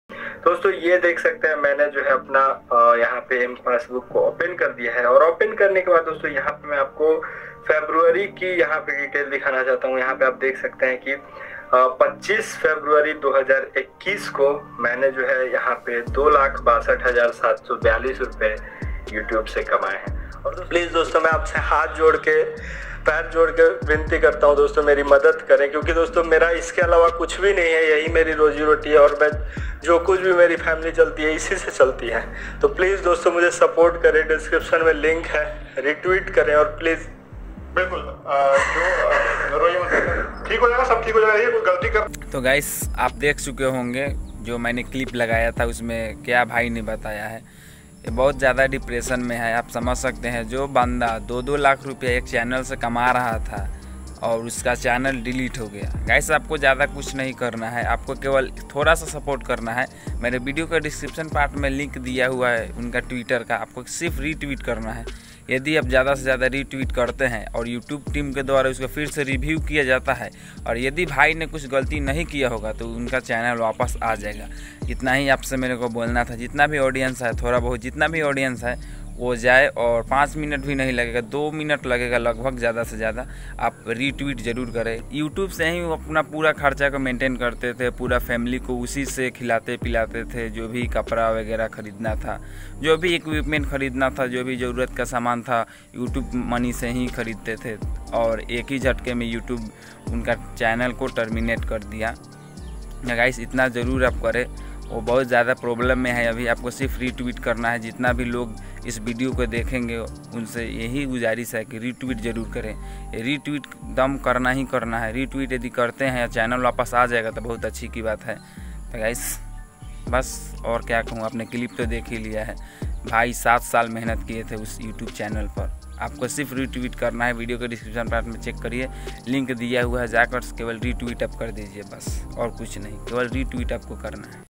Monthly income 2 lakh था और subscriber 4 million | चैनल delete होगया 😥 | please support 🙏🙏
technical Baba Nitya delete ho gaya hai please support kare bhai ko 🙏